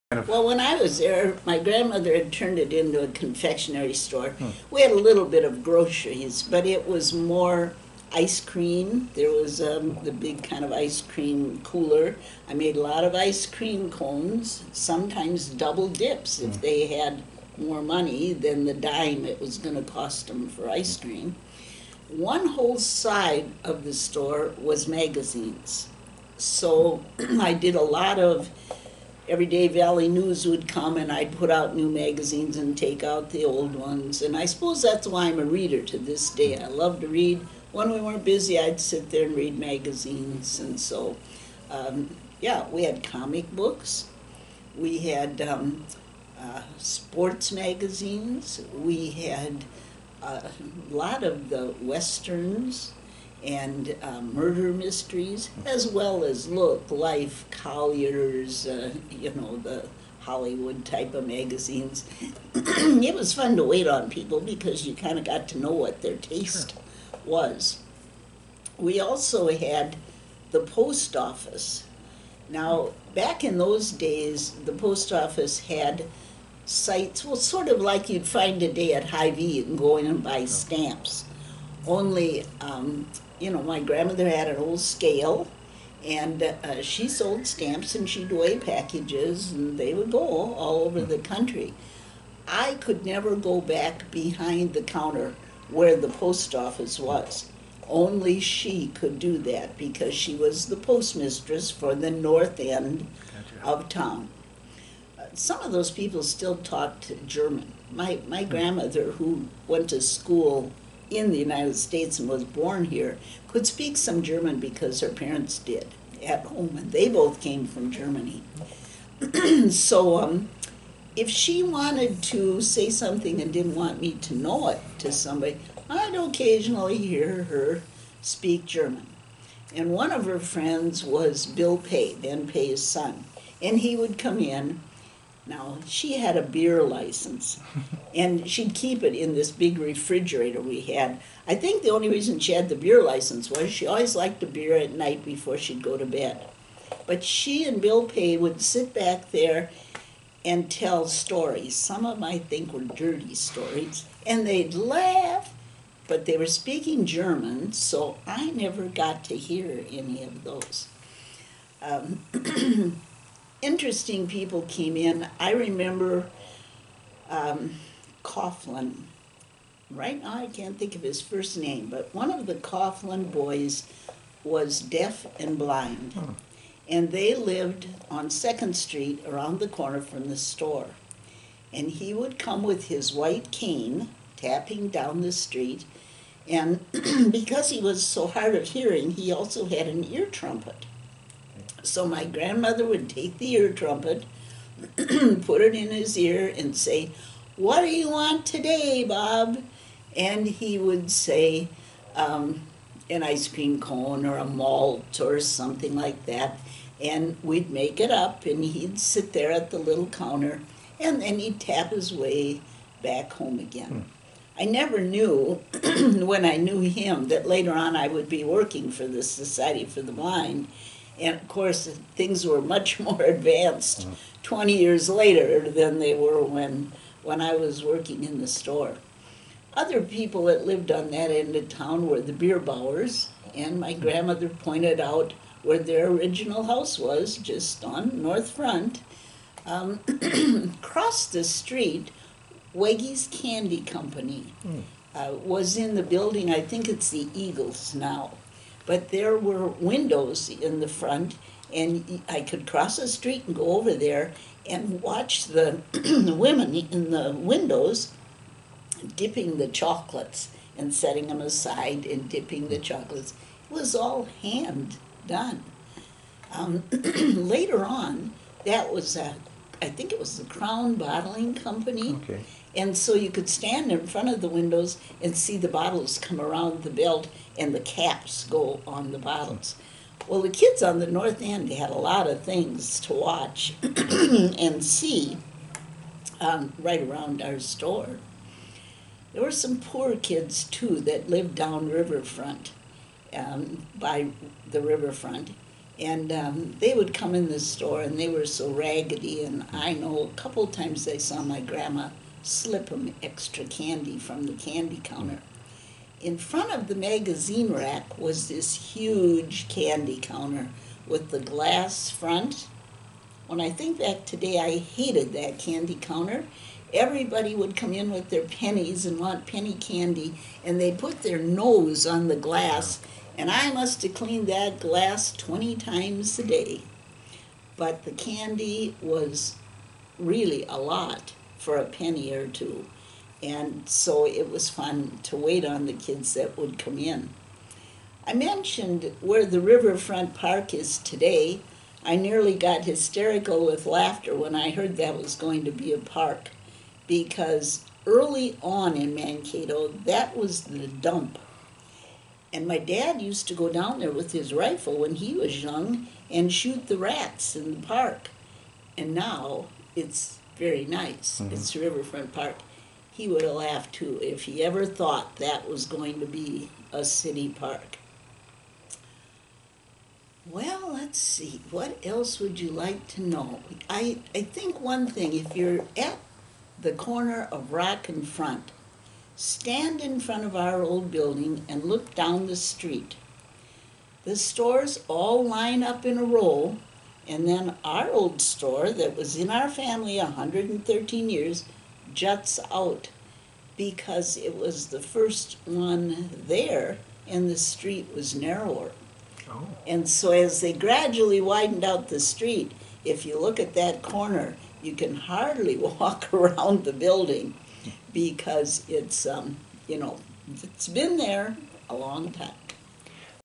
Life at the store 3
Mankato native Georgia "Jo" Schultz speaks on growing up in the river town and her family's business endeavors.